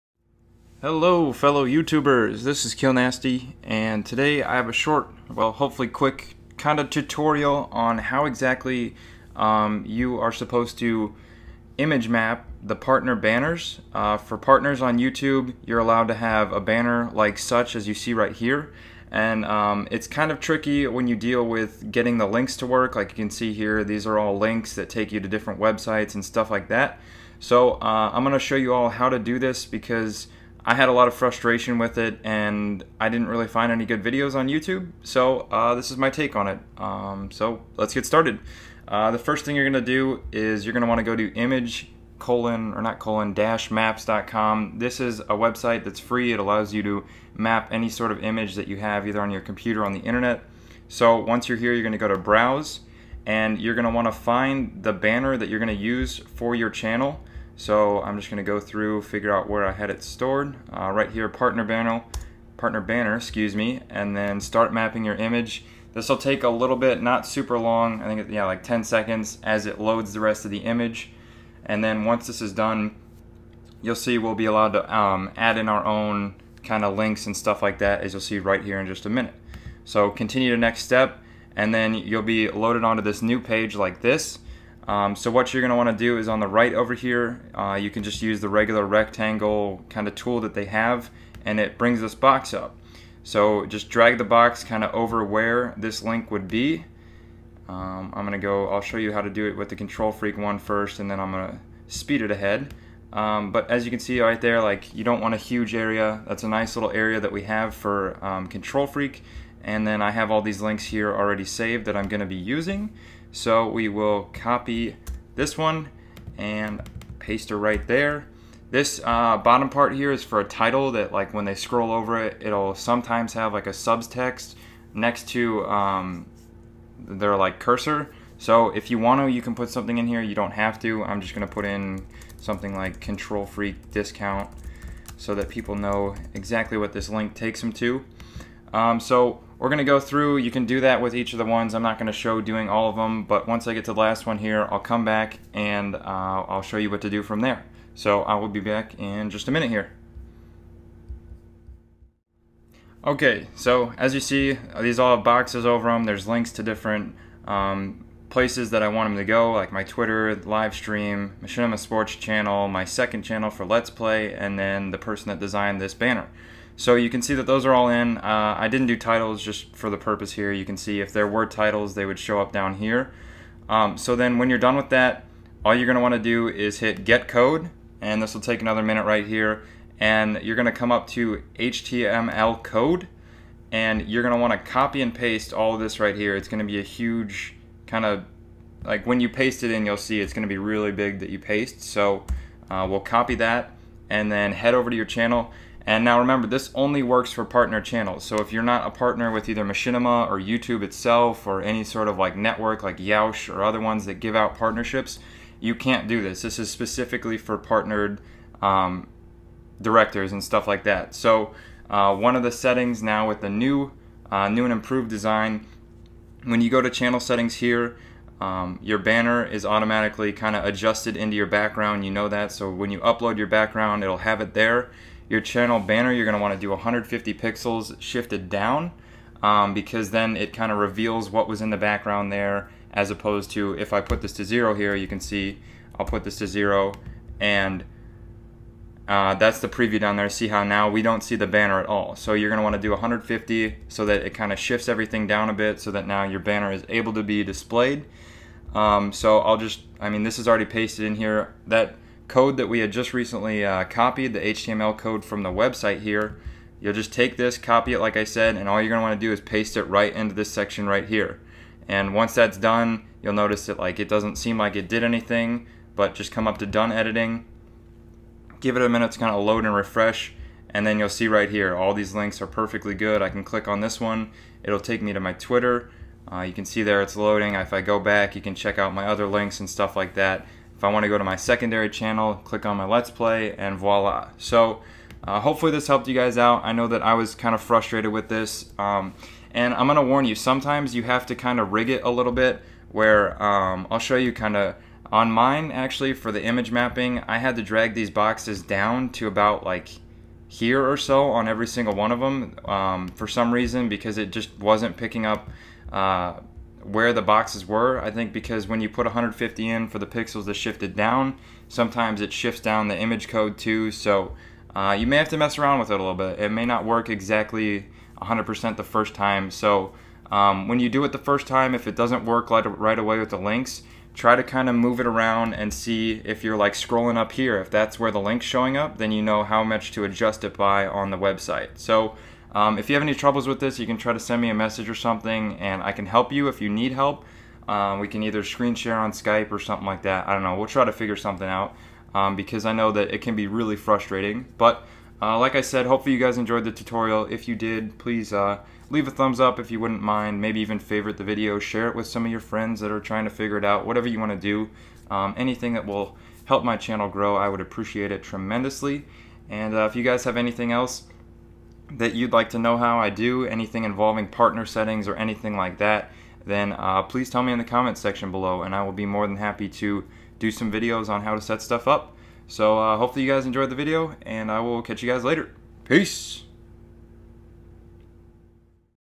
YouTube Guide: Partner Banner Linking | How to Create Image Map | New Layout/Design
I was having a lot of trouble learning how to create the Image Map required for Partner Banners to make the links clickable and stuff, so I decided to do a video showing how to do it to hopefully save you all some frustration!